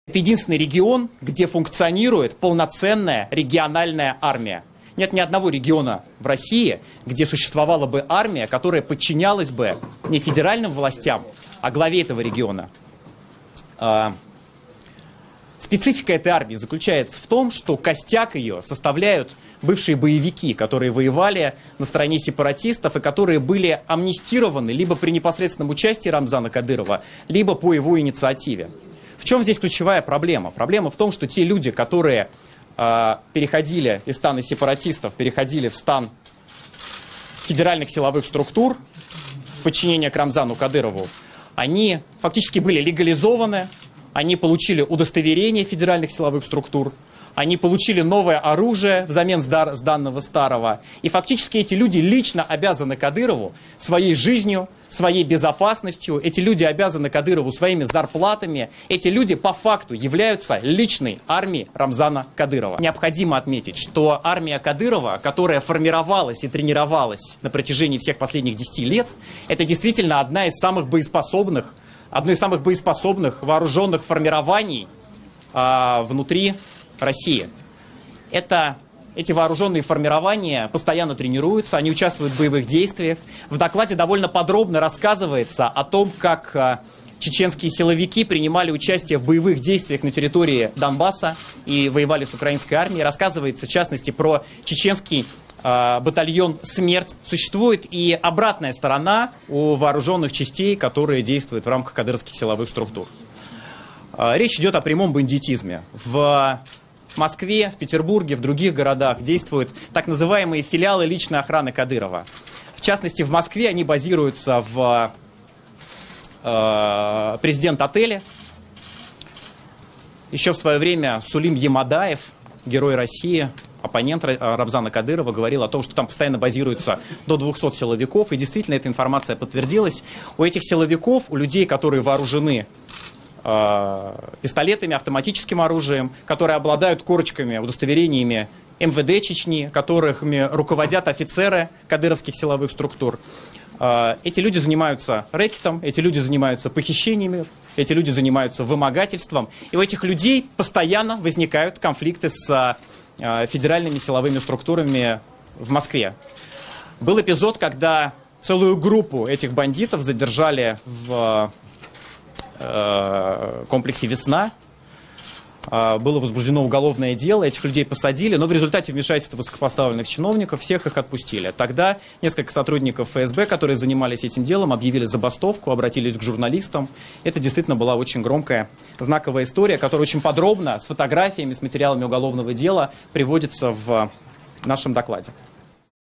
Ільля Яшын аб прыватнам войску Кадырава
Па дадзеных Ільлі Яшына, колькасьць падразьдзяленьняў, якія падпарадкоўваюцца асабіста Рамзану Кадыраву, складае каля 30 тысяч чалавек.